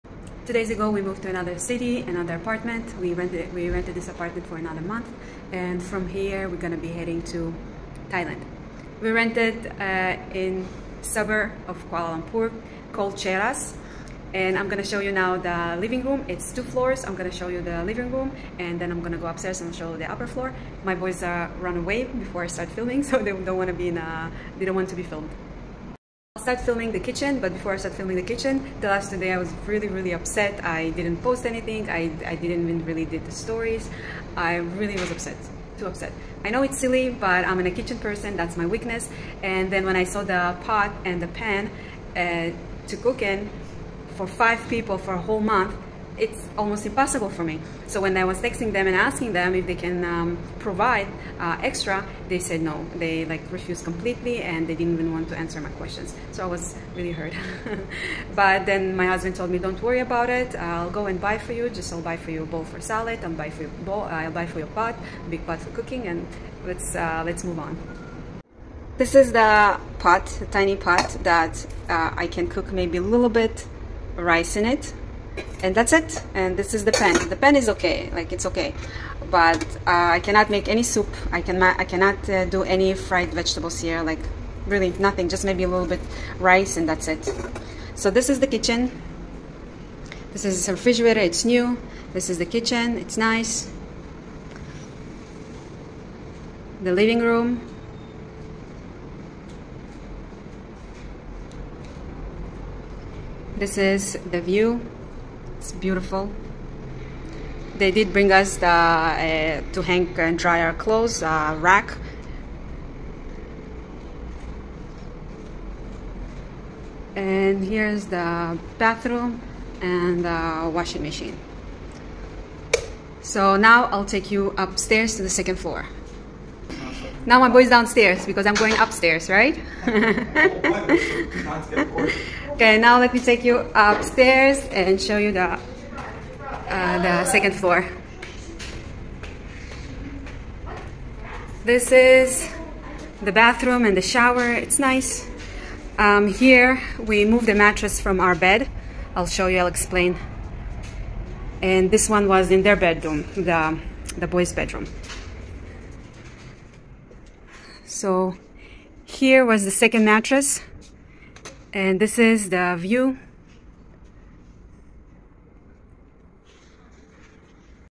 Our New Apartment For The Next Month, Part 1!
We moved from Melaka to Kuala Lumpur for our last month in Malaysia!

✈️ FOLLOW @tatianna_yougottalove
If you Love Traveling & Food!

Tatianna Yougottalove 💕